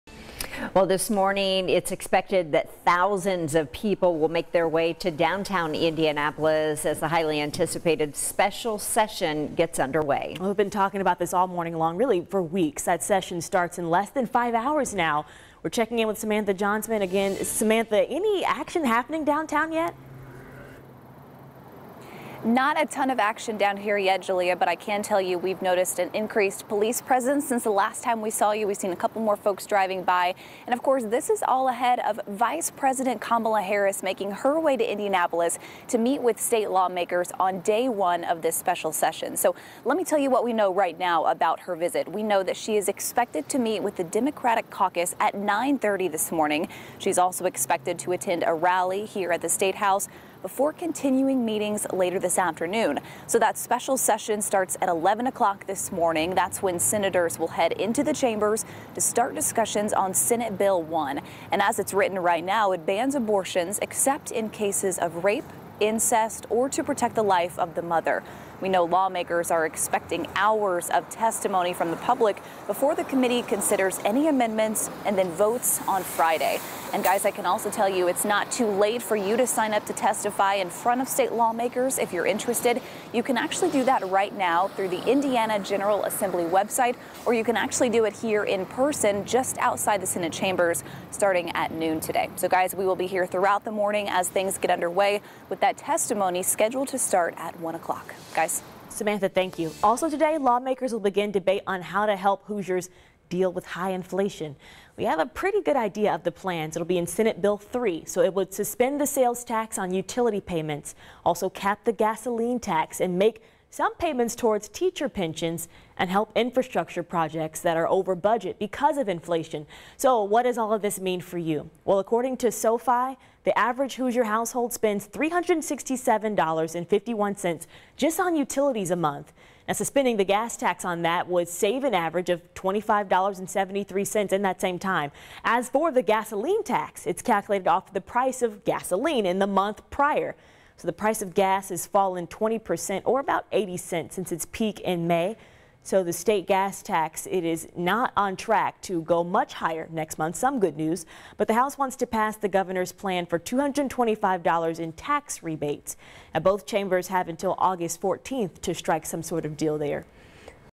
Preview of the 1st week of the special session on the abortion ban and tax refund
Indiana lawmakers will start work during a special session on Monday, July 25 to consider abortion ban legislation and how to give Hoosiers a tax break given the state's surplus.